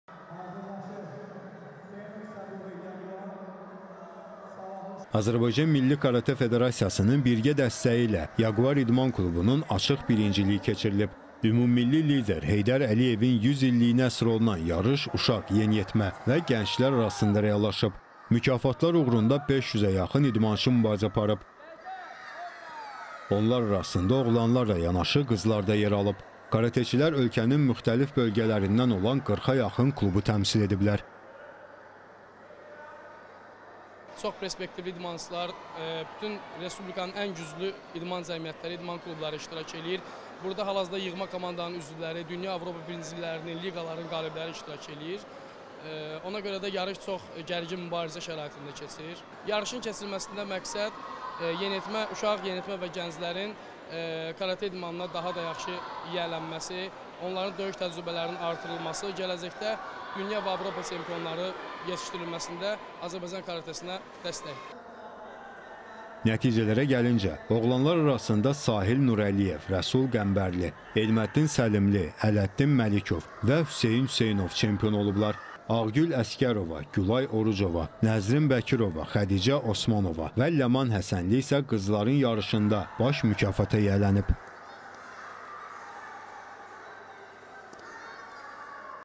Karate üzrə açıq birincilik keçirilib
Yarış uşaq, yeniyetmə və gənclər arasında reallaşıb